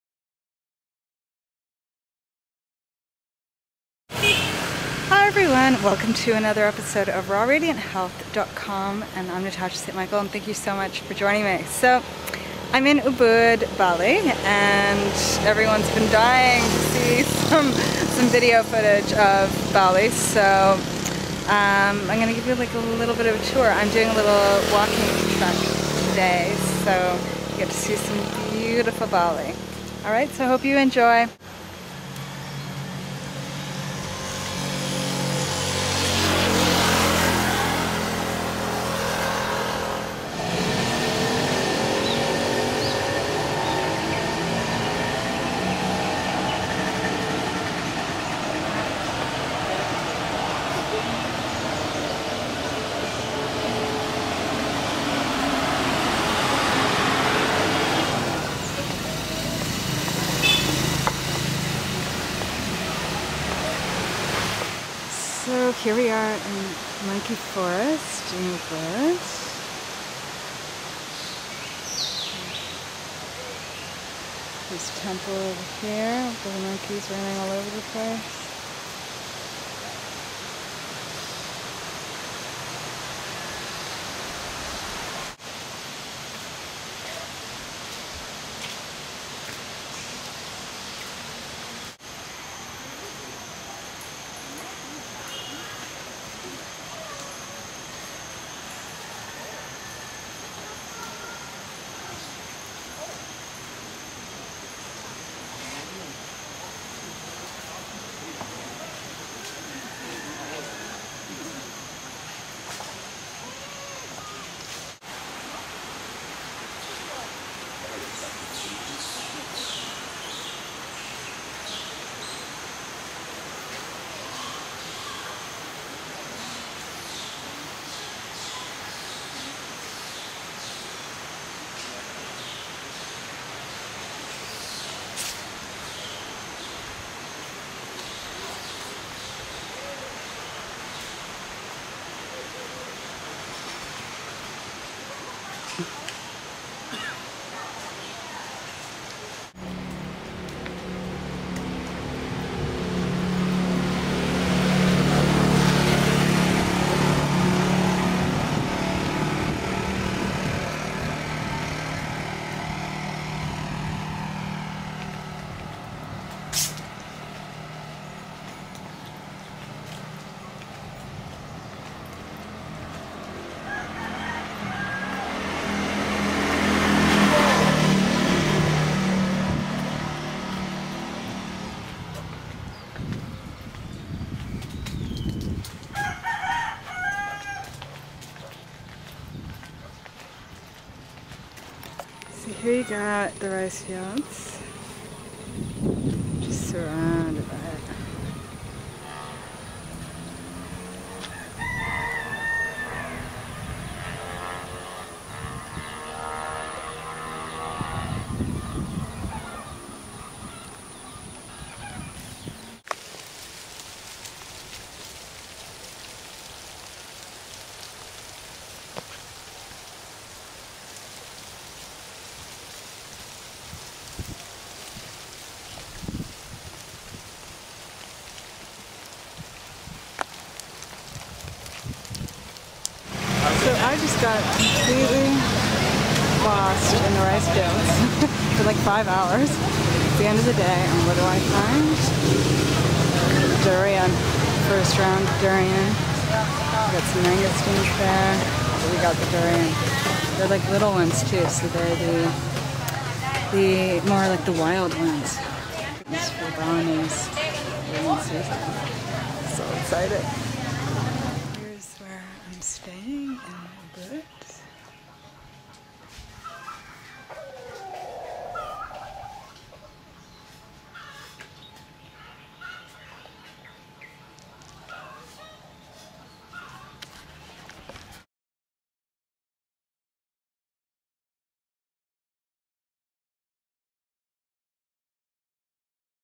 Ubud, Bali, Ep316
I've been receiving many requests for some videos on Bali and my travels-- so here's some video of Ubud!

To learn more about the Raw Food Diet, Weight Loss, Consultations/Coaching, Juice Fasting and the 7 Day Challenge, go